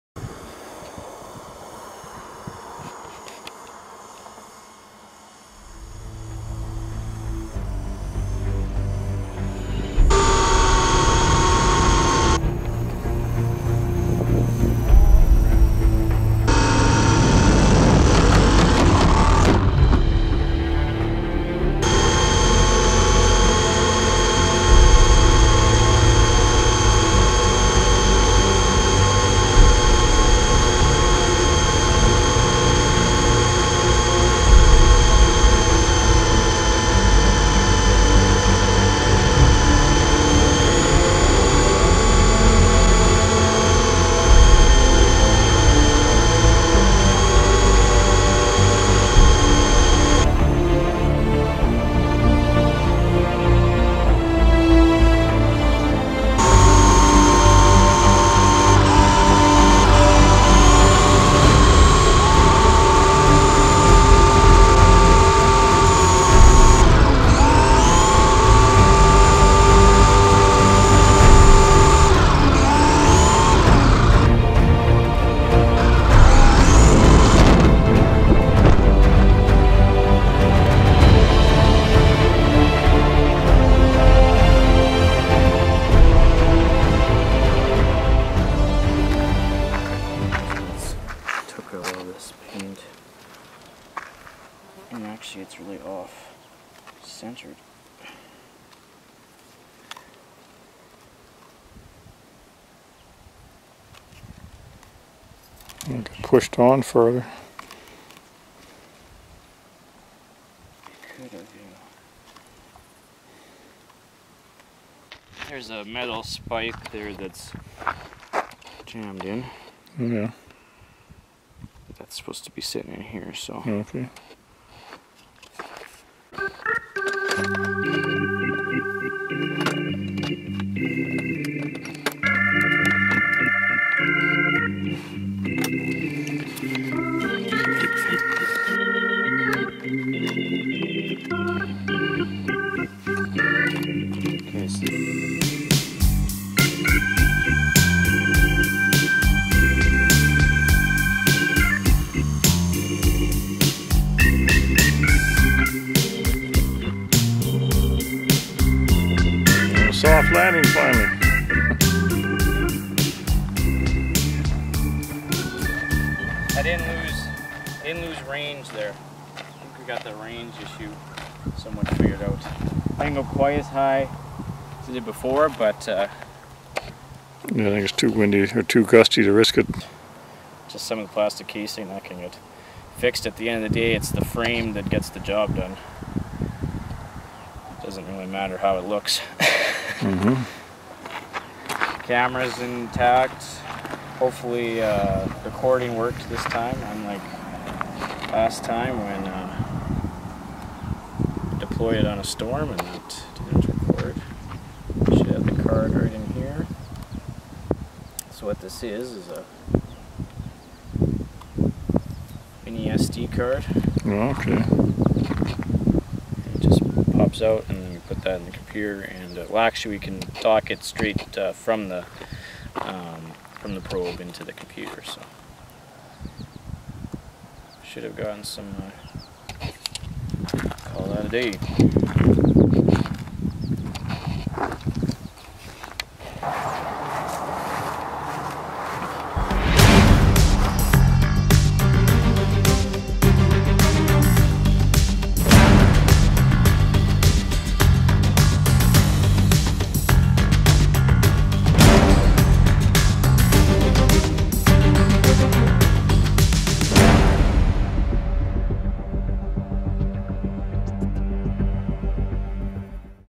Flying The Probe-Storm Hunters 2013
The SKWX Team tests out their new RC Camera Probe for the storm season.  All new web series, Storm Hunters, coming this fall to SaskatchewanWeather.ca!!